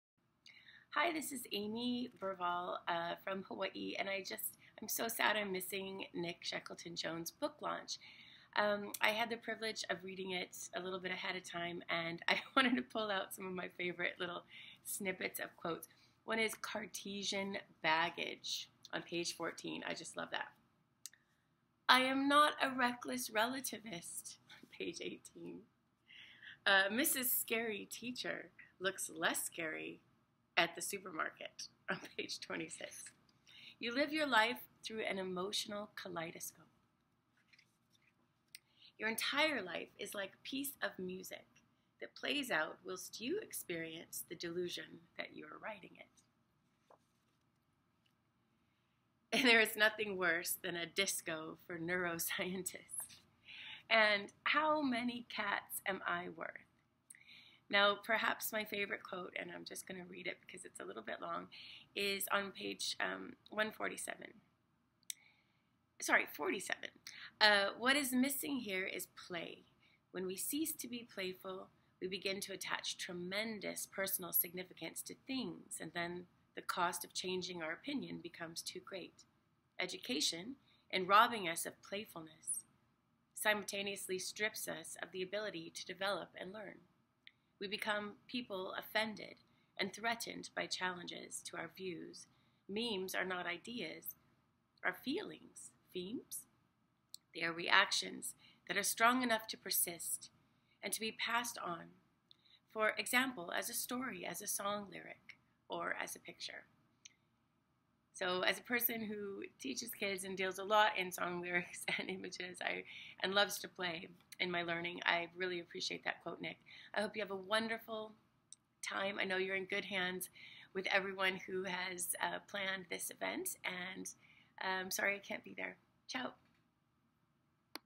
Congrats on How People Learn Book Launch, Nick Shackleton-Jones
I couldn't be at the book launch in London, but highly endorse this new book: How People Learn (Kogan Page, 2019) by Nick Shackleton-Jones